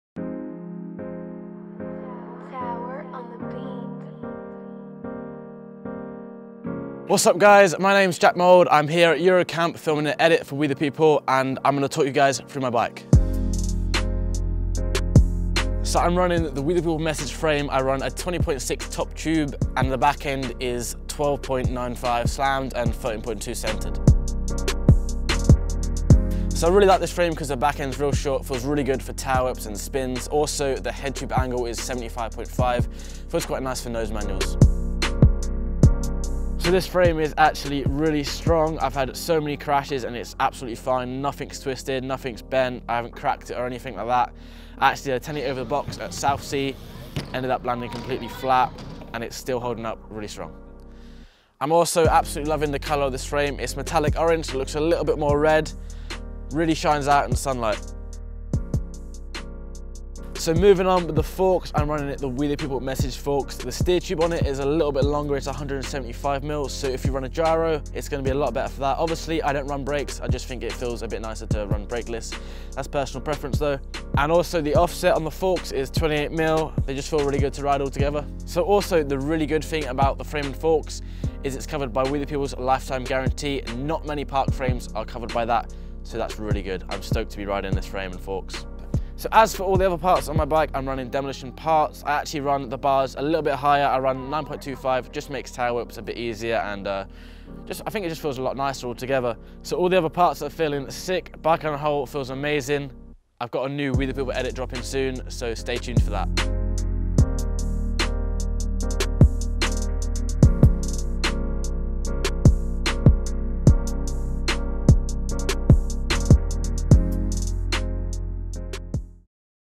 WETHEPEOPLE BMX: Jack Mould MESSAGE Bike Check
Jack Mould has been out at Eurocamp working on a new video project. Whilst we were waiting for the ramps to dry, we decided to take a closer look at Jack's ride and find out more about why our MESSAGE line of parts are perfect for a new-school skatepark machine like Jack.

Filmed & Edited by Callum Earnshaw.